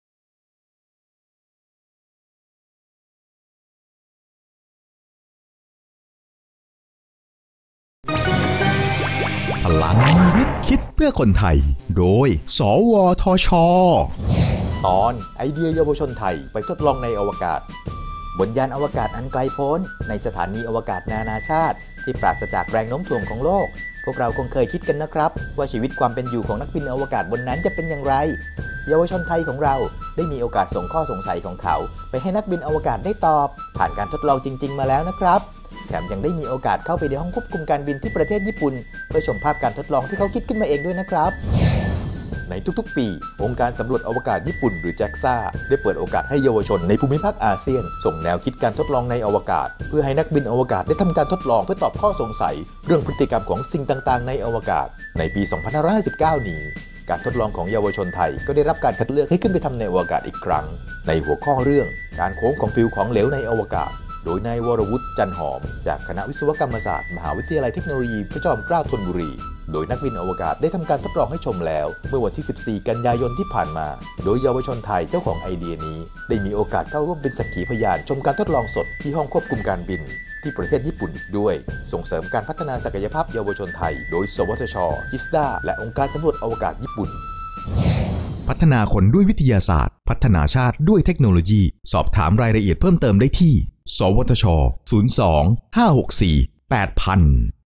พลังวิทย์ คิดเพื่อคนไทย ตอน ไอเดียเยาวชนไทย ไปทดลองในอวกาศ
บนยานอวกาศอันไกลโพ้น ในสถานีอวกาศนานาชาติที่ปราศจากแรงโน้มถ่วงของโลก พวกเราคงเคยคิดกันว่า ชีวิตความเป็นอยู่ของนักบินอวกาศบนนั้นจะเป็นอย่างไร เยาวชนไทย ได้มีโอกาสส่งข้อสงสัยของเขา ไปให้นักบินอวกาศได้ตอบ ผ่านการทดลองจริงๆ มาแล้ว แถมยังได้มีโอกาสเข้าไปในห้องควบคุมการบินที่ประเทศญี่ปุ่น เพื่อชมภาพการทดลองที่พวกเขาคิดขึ้นมาเอง ด้วยการสนับสนุนของ สวทช. จิสด้า และ องค์การสำรวจอวกาศญี่ปุ่น

ในทุกๆ ปีองค์การสำรวจอวกาศญี่ปุ่น หรือ Jaxa ได้เปิดโอกาสให้เยาวชนในภูมิภาคอาเซียน ส่งแนวคิดการทดลองในอวกาศ เพื่อให้นักบินอวกาศได้ทำการทดลอง เพื่อตอบข้อสงสัยเรื่องพฤติกรรมของสิ่งต่างๆ ในอวกาศ ในปี 2559 นี้ การทดลองของเยาวชนไทย ก็ได้รับการคัดเลือกให้ขึ้นไปทำในอวกาศอีกครั้ง ในหัวข้อเรื่อง การโค้งของผิวของเหลวในอวกาศ โดย นายวรวุฒิ จันทร์หอม จากคณะวิศวกรรมศาสตร์ มหาวิทยาลัยเทคโนโลยีพระจอมเกล้าธนบุรี โดยนักบินอวกาศได้ทำการทดลองให้ชมแล้ว เมื่อวันที่ 14 กันยายน ที่ผ่านมา โดยเยาวชนไทยเจ้าของไอเดียนี้ ได้มีโอกาสเข้าร่วมเป็นสักขีพยานชมการทดลองสด ที่ห้องควบคุมการบิน ที่ประเทศญี่ปุ่นอีกด้วย